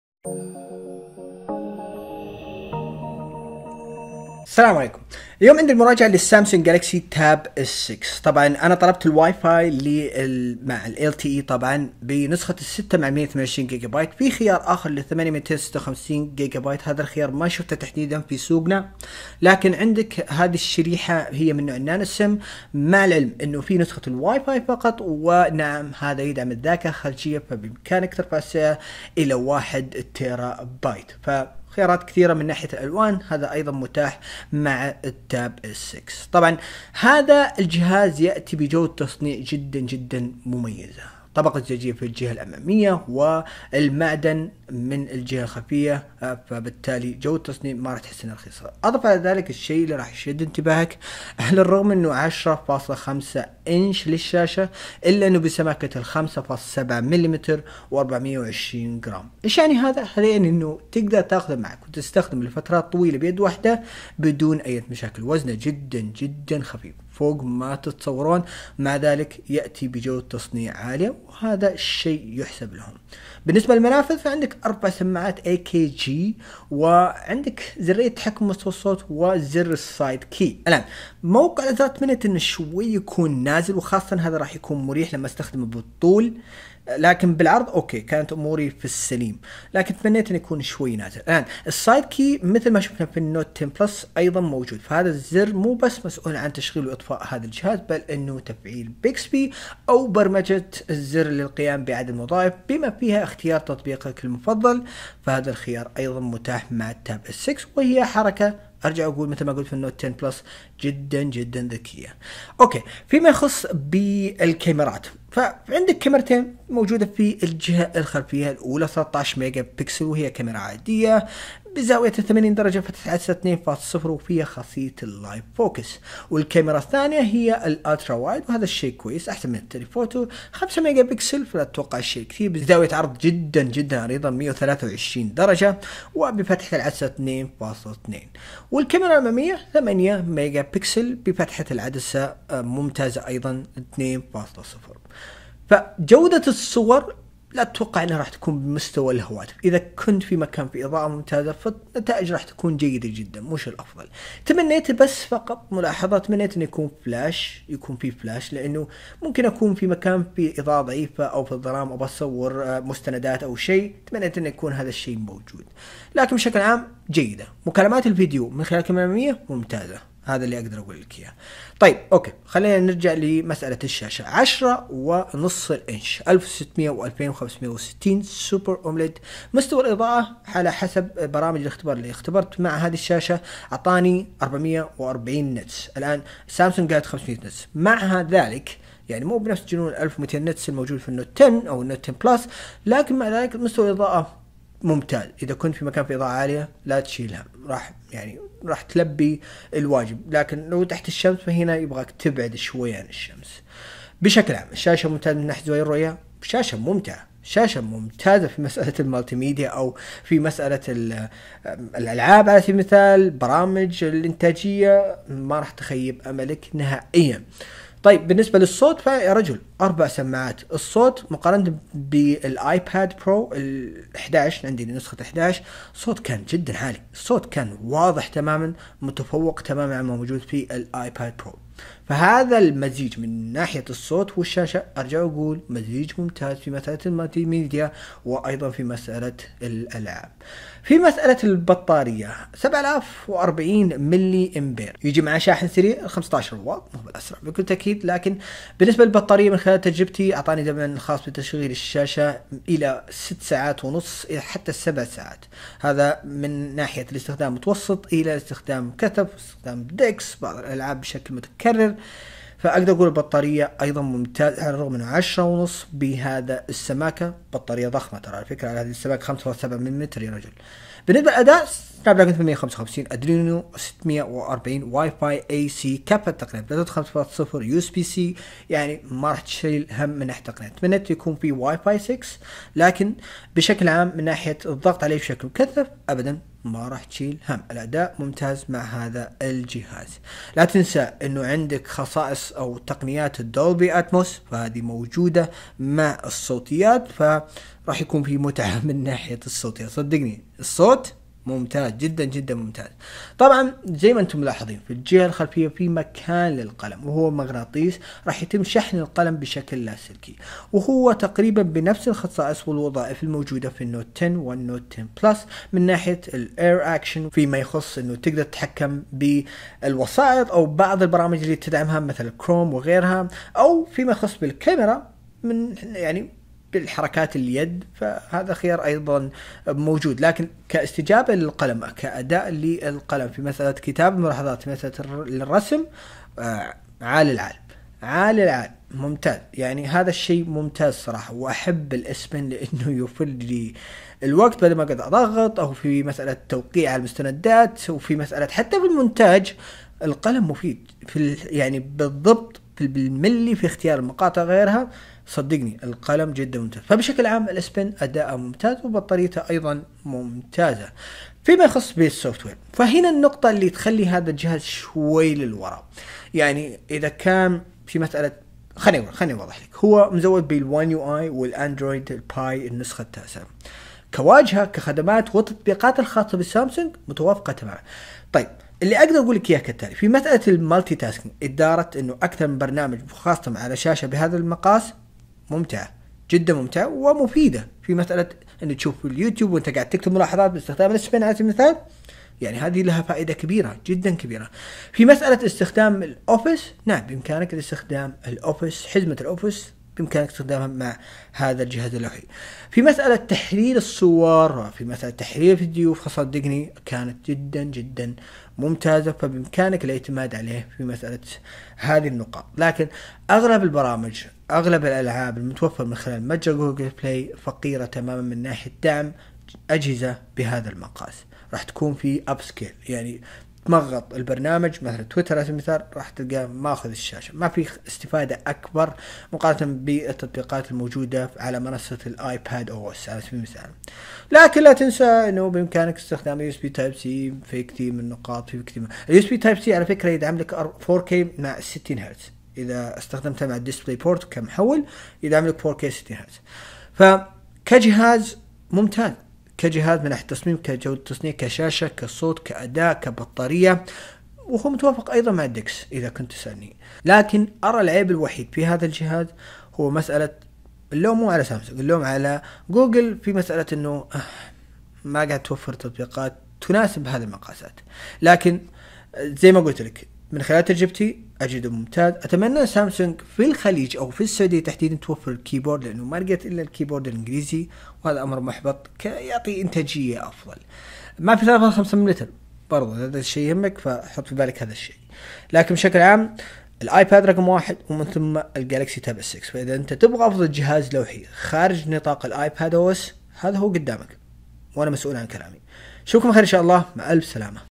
أفضل جهاز لوحي بنظام الأندرويد Samsung Galaxy Tab S6
في هذا الفيديو نقدم لكم مراجعة للجهاز اللوحي Samsung Galaxy Tab S6.إذا عجبك الفيديو لاتنسى تضغط على زرّ اللايك والأشتراك في القناة ونشر الفيديو وكل الشكر لكم.

سعر الجهاز Samsung Galaxy Tab S6 يبدأ نت 649 دولار أمريكي مع العلم أن النسخة التي تم مراجعتها تم شرائها بـ 2750 ريال سعودي نسخة WiFi+LTE  بسعة 128 جيجا مع 6 جيجا للرام

تقديم: